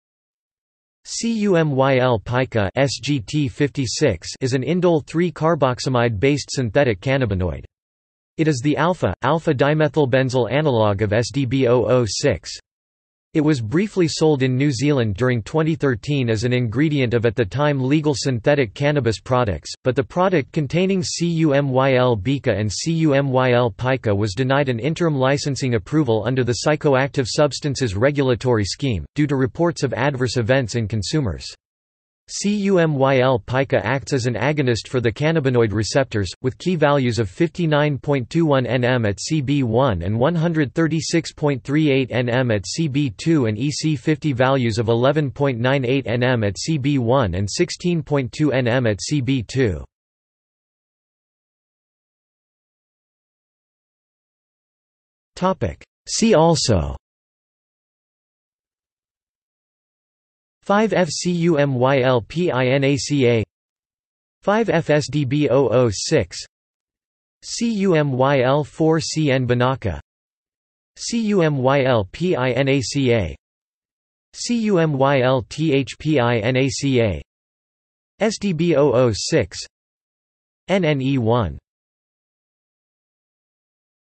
CUMYL-PICA | Wikipedia audio article
This is an audio version of the Wikipedia Article:
CUMYL-PICA

00:00:51 undefined

- Socrates

SUMMARY
CUMYL-PICA (SGT-56) is an indole-3-carboxamide based synthetic cannabinoid. It is the α,α-dimethylbenzyl analogue of SDB-006. It was briefly sold in New Zealand during 2013 as an ingredient of at the time legal synthetic cannabis products, but the product containing CUMYL-BICA and CUMYL-PICA was denied an interim licensing approval under the Psychoactive Substances regulatory scheme, due to reports of adverse events in consumers. CUMYL-PICA acts as an agonist for the cannabinoid receptors, with Ki values of 59.21 nM at CB1 and 136.38 nM at CB2 and EC50 values of 11.98 nM at CB1 and 16.2 nM at CB2.